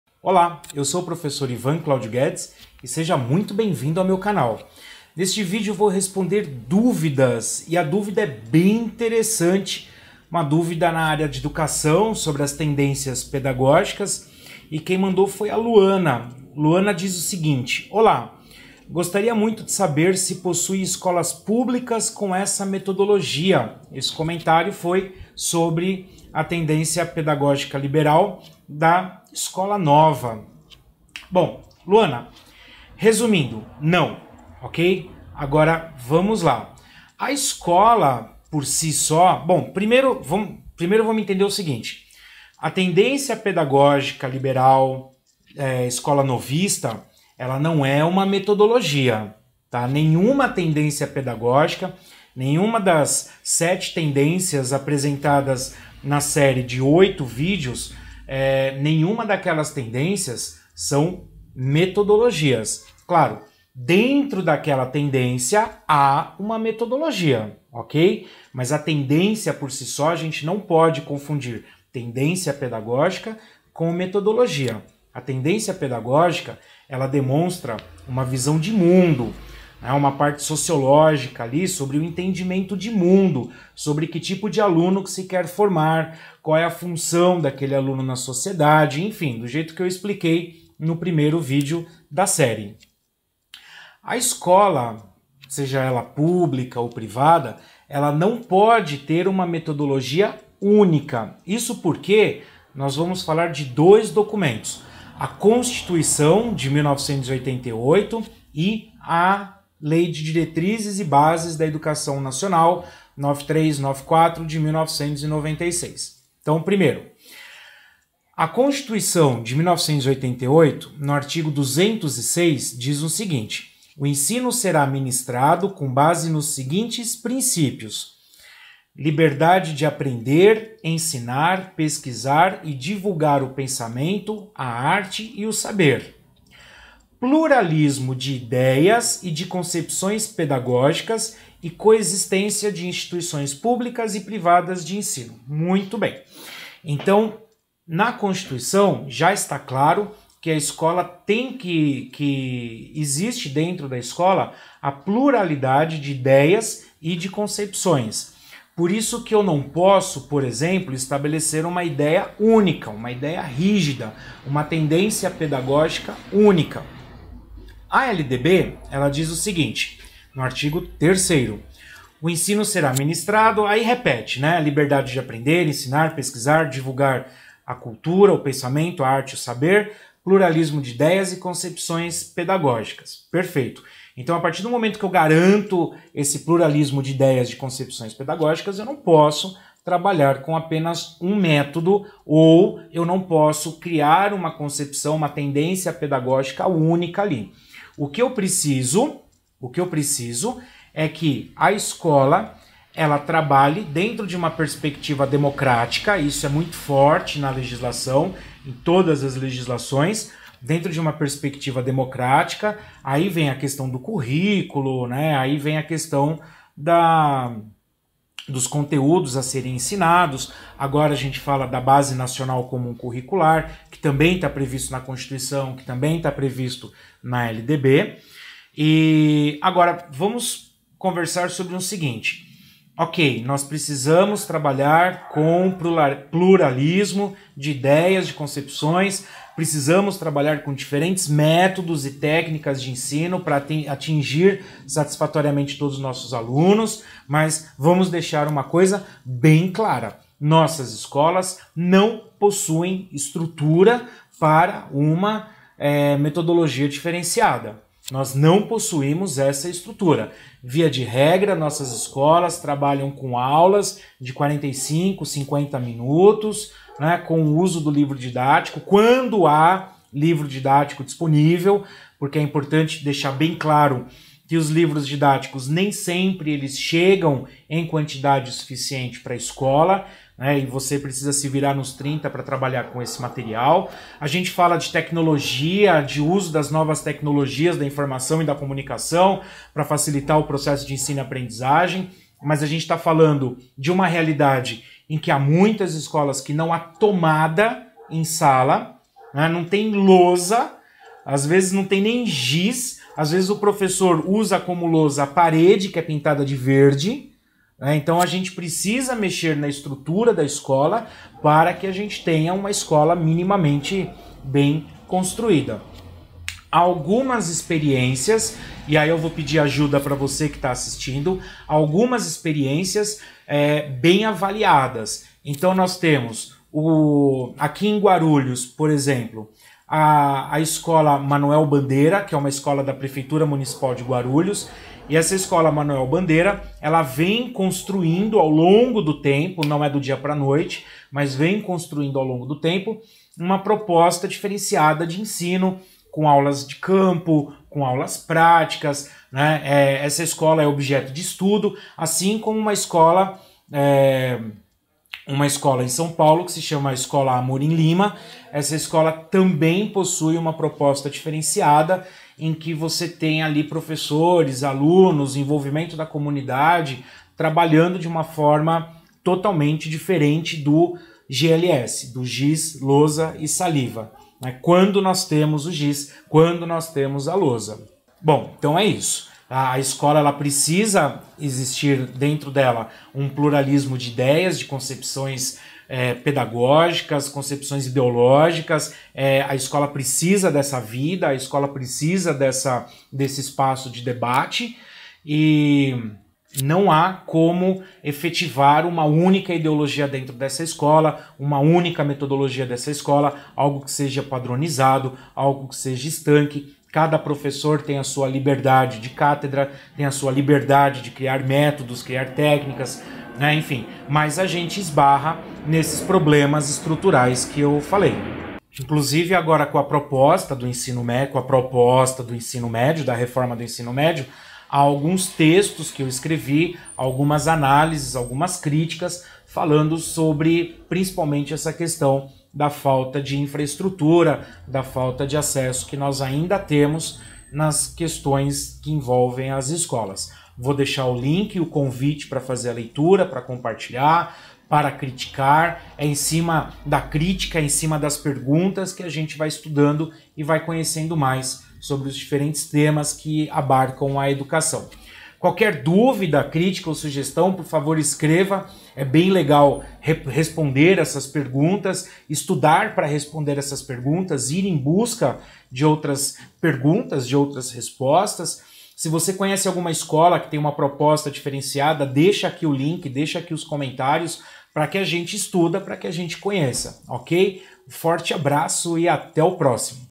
A ESCOLA PODE TER IDEOLOGIA?
A escola pode ter uma única ideologia? ou um único método?
Neste vídeo eu respondo sobre essa questão com base na Lei de Diretrizes e Bases da Educação nacional LDBEN 9.394/1996 e com base na ConstituiçãoFederal.